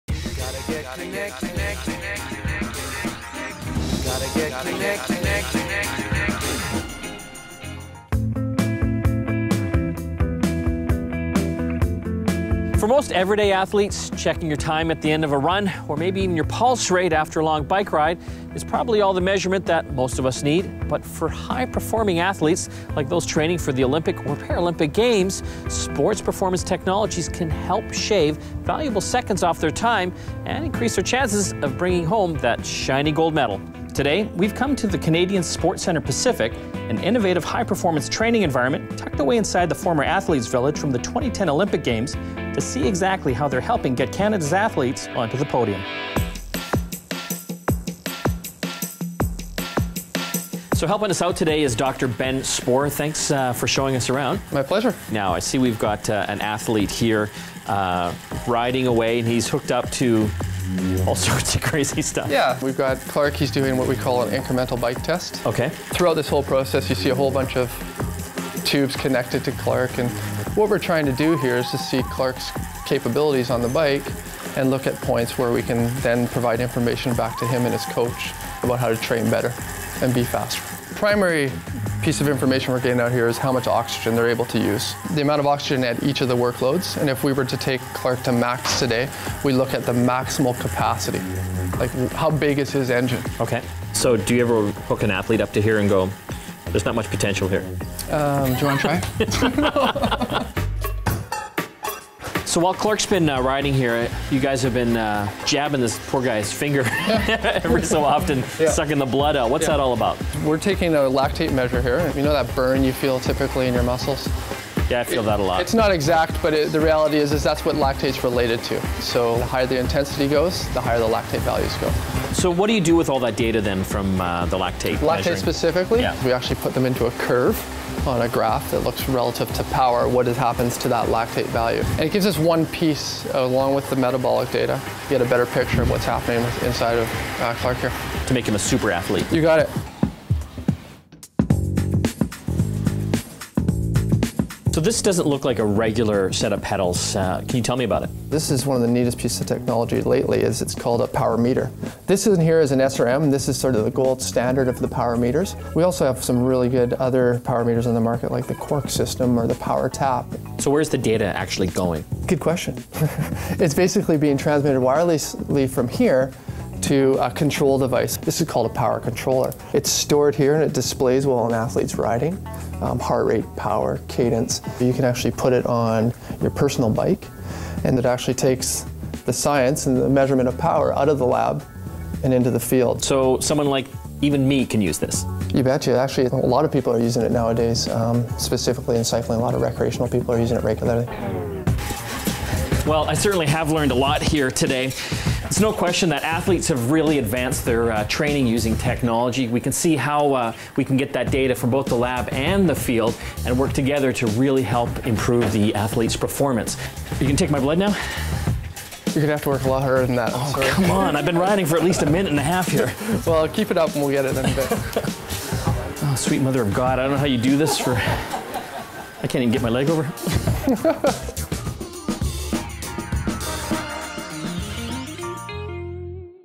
Technology at Canadian Sport Centre Pacific
At Canadian Sport Centre Pacific in the Olympic Village in Vancouver, Mike checks out how sports performance technology helps high-performing athletes train. See how tests like incremental bike tests, and lactate measurements help provide information back to the athlete and help them achieve optimal training. Mike also learns about a unique technology called the PowerMeter by SRM, sending key training data directly to the athletes.